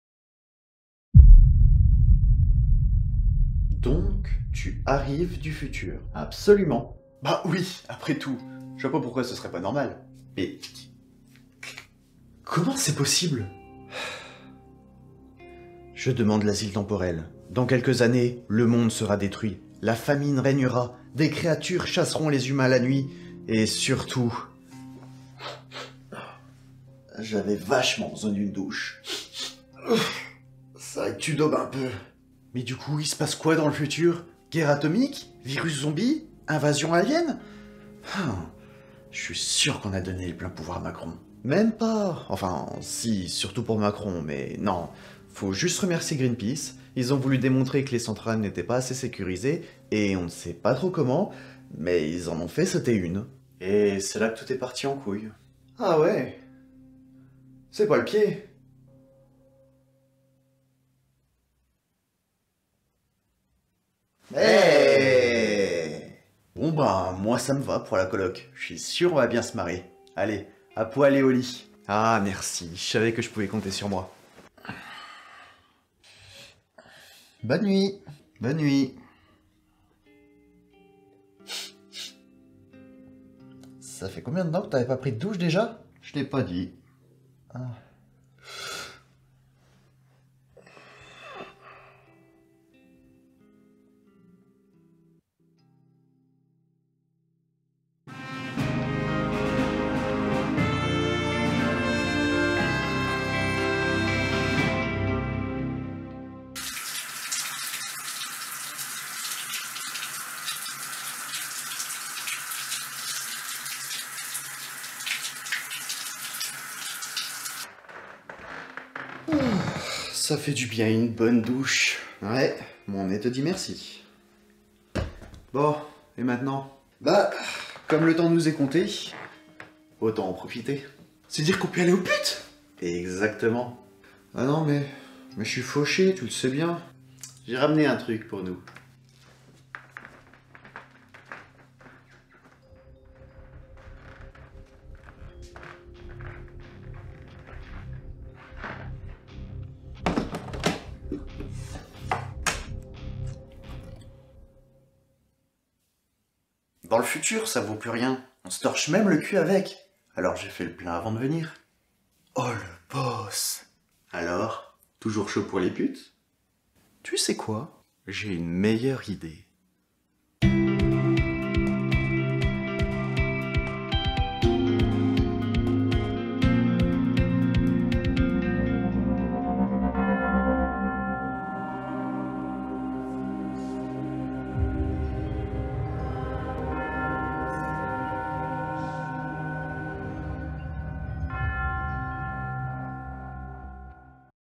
Le Double - Back to the Past - Episode 2
Dans ce deuxième épisode, Ekinoxx du Futur remporte un petite surprise qui va changer la vie d'Ekinoxx du Présent !

Production :  @Ekinoxxstudio
Ekinoxx :  @Ekinoxxstudio
Réalisation/Scénario :  @Ekinoxxstudio et @lloegyrfilm6041
Cadrage : @lloegyrfilm6041
MUA : Elodie Antoine
Musiques : Arnaud Pierson